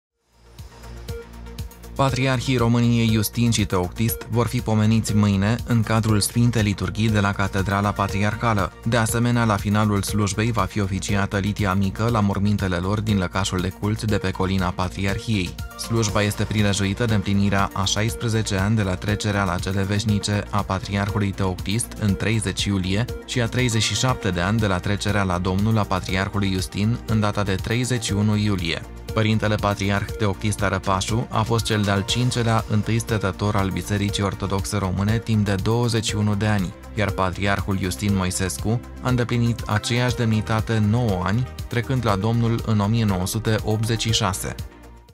Patriarhii Iustin și Teoctist vor fi pomeniți la Catedrala Patriarhală
Patriarhii României Iustin și Teoctist vor fi pomeniți mâine în cadrul Sfintei Liturghii de la Catedrala Patriarhală. De asemenea, la finalul slujbei va fi oficiată litia mică la mormintele lor din lăcașul de cult de pe Colina Patriarhiei. Slujba este prilejuită de împlinirea a 16 ani de la trecerea la cele veșnice a Patriarhului Teoctist, în 30 iulie, și a 37 de ani de la trecerea la Domnul a Patriarhului Iustin, în data de 31 iulie.  Părintele Patriarh Teoctist Arăpașu a fost cel de-al 5-lea Întâistător al Bisericii Ortodoxe Române timp de 21 de ani, iar Patriarhul Iustin Moisescu a îndeplinit aceeași demnitate 9 ani, trecând la Domnul în 1986.